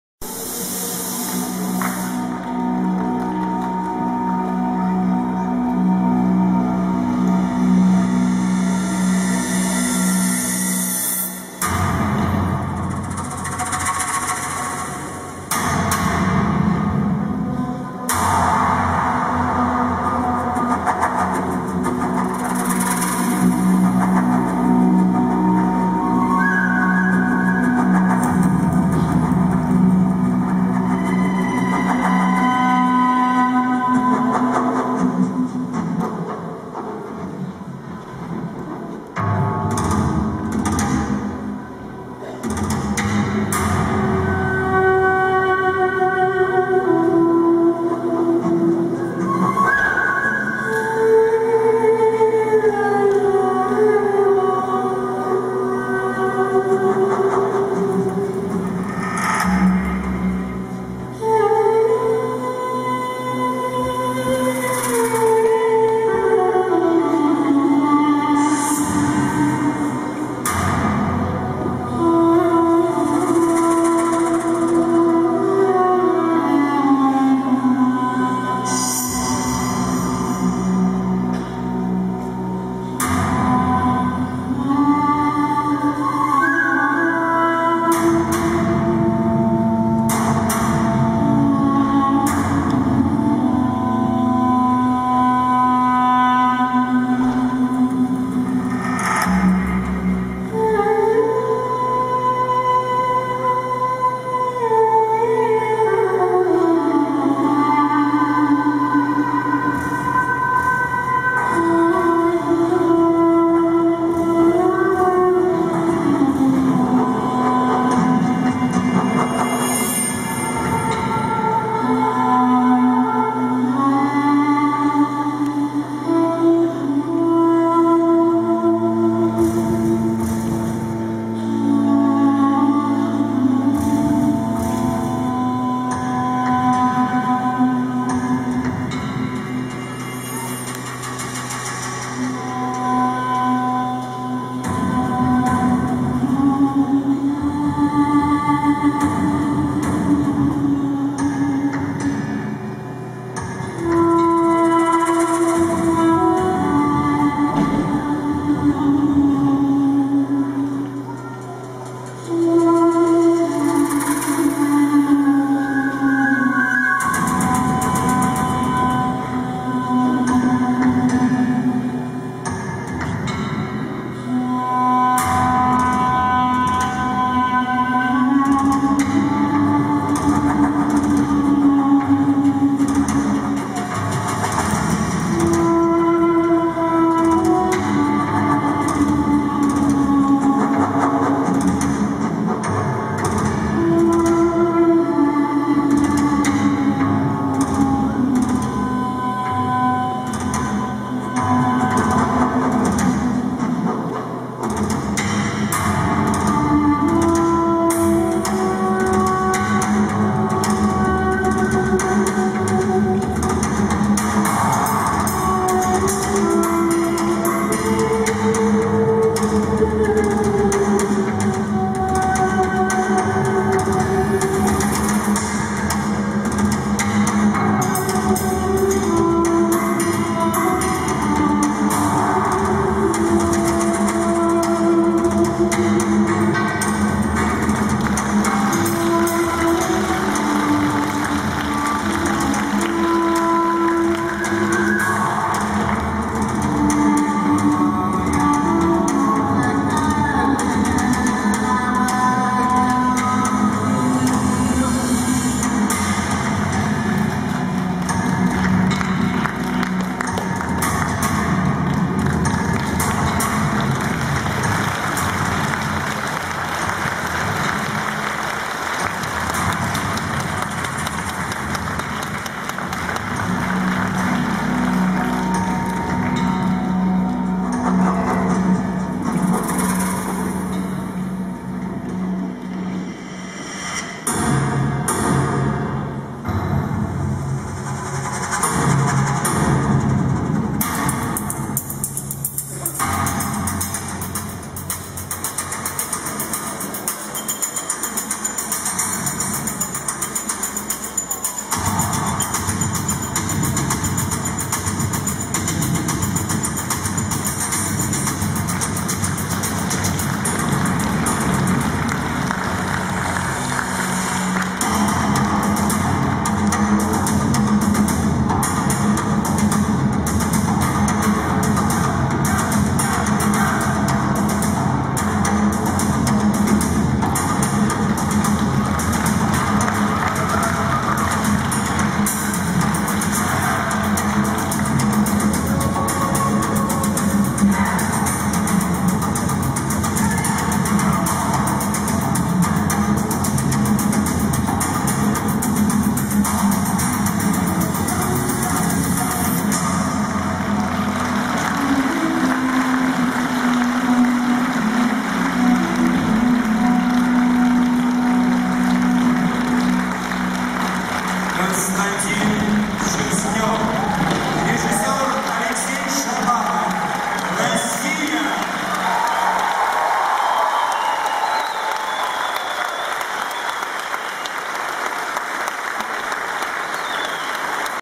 Roue Cyr "Shaman" рейнское колесо - Konstantin Sherstnev - Каталог артистов
Roue Cyr "Shaman" рейнское колесо на Цирковом фестивале в Санкт-Петербурге. 2010 год.